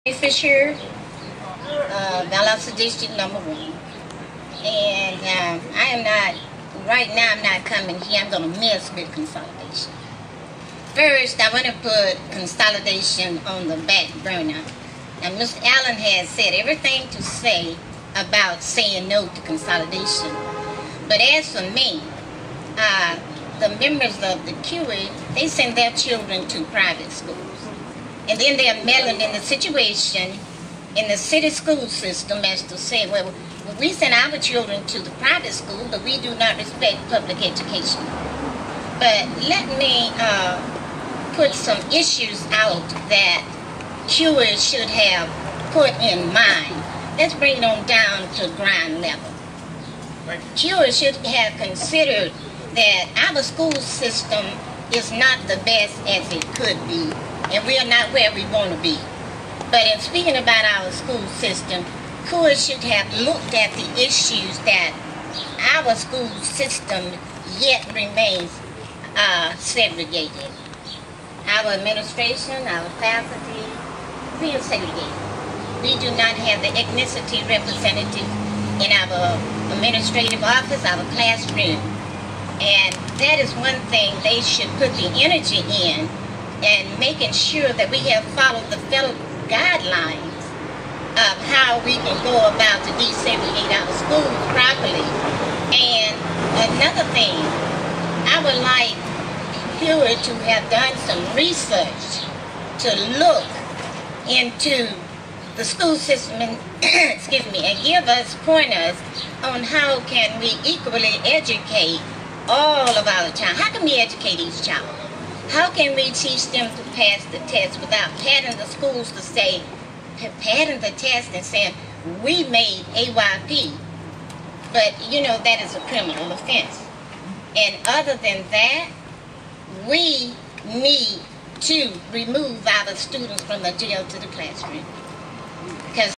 CUEE members send their children to private schools --Annie Fisher, FVCS, 7 July 2011
Valdosta school board member Annie Fisher spoke.
She pointed out CUEE members sent their chidren to private schools
and now they're meddling in public education.
She listed some real issues, such as
Valdosta city schools remain segregated,
focussing on tests just to meet AYP,
and we need to remove students from the prison to the classroom.
No school consolidation
Press Conference, Friends of Valdosta City Schools (FVCS),